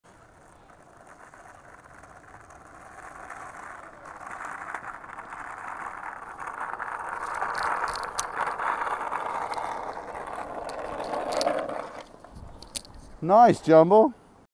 Skateboarding Norwich Terrier
Our Norwich Terrier tried skateboarding today and loved it! Watch him tear it up!